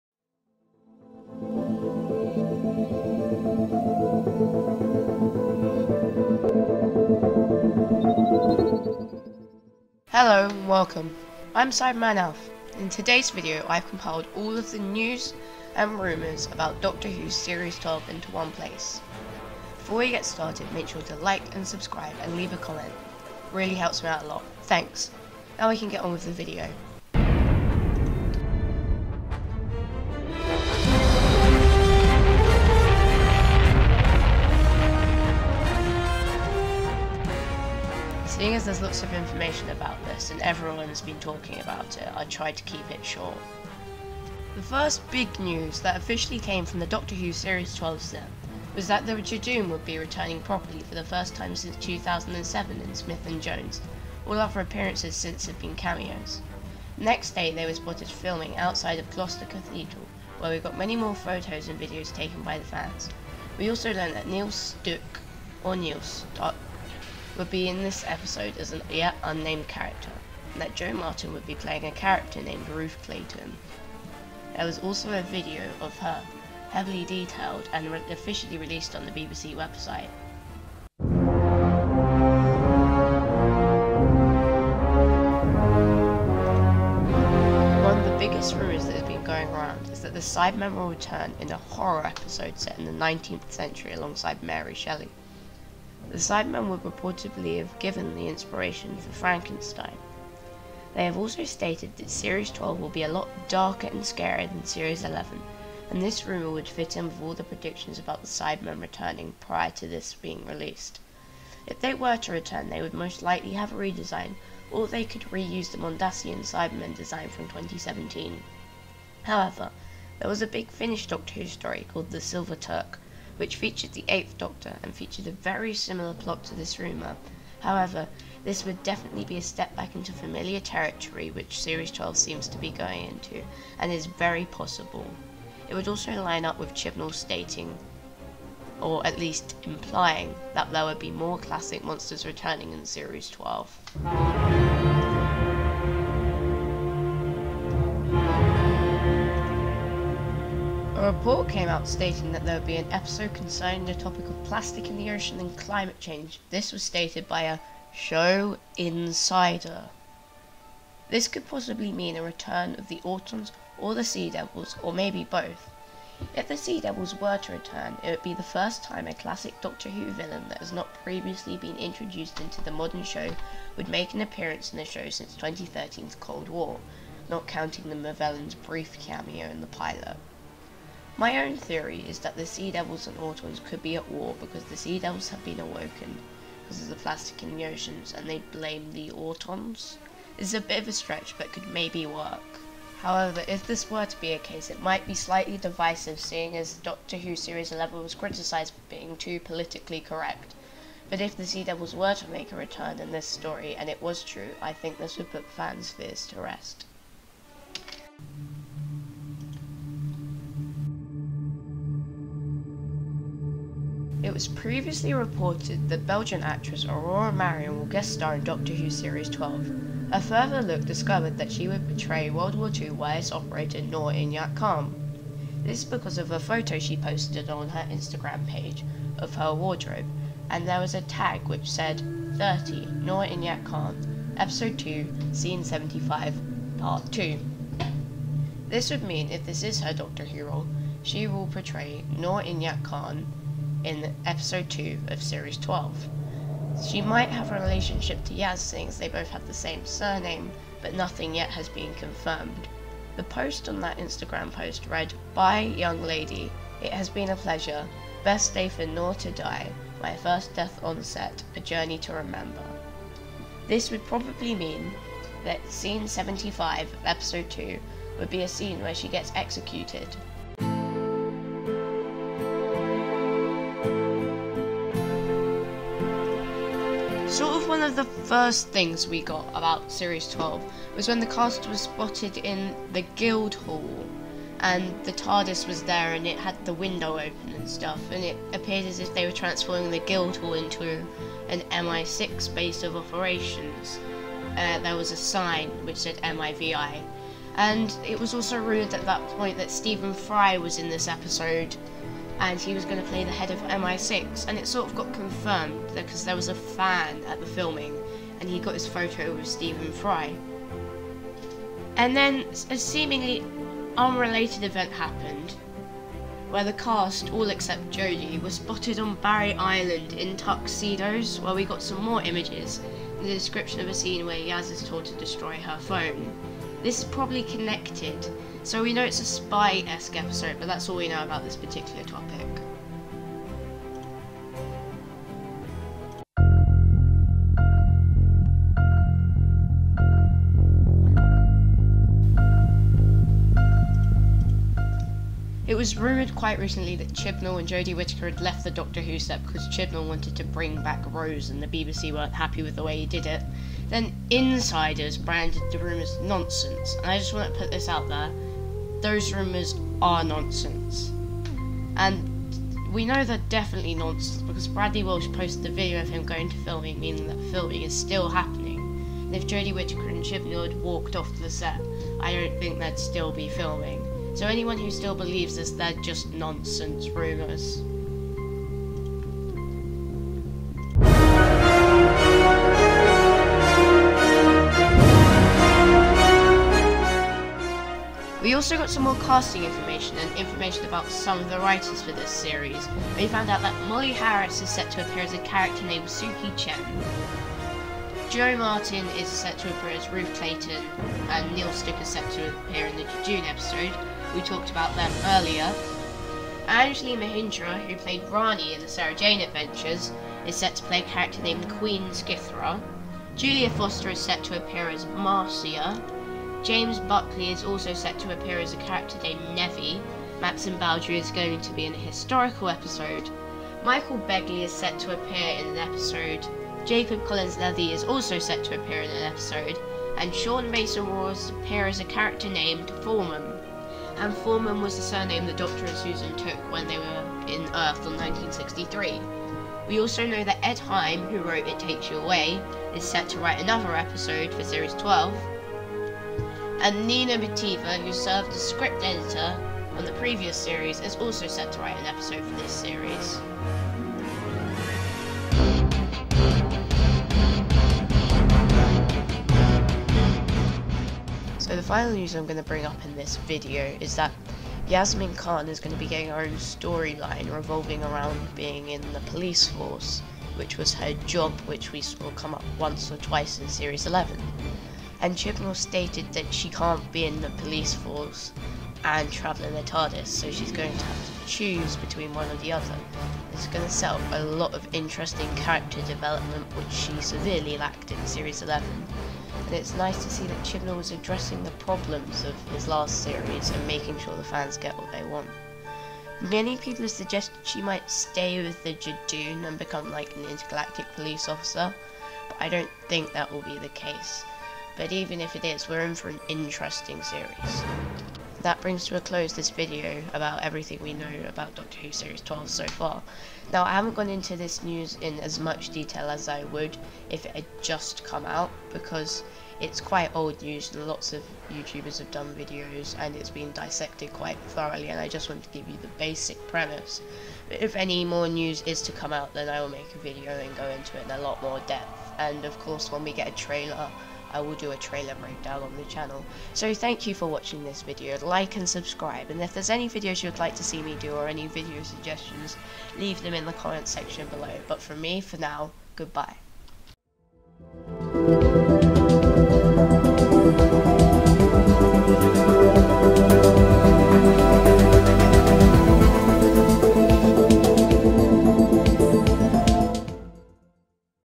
Doctor Who Series 12 - Everything We Know So Far
Hello, and welcome I'm Cybermanalf. This video is just going through all the news and rumours we know about Doctor Who Series 12.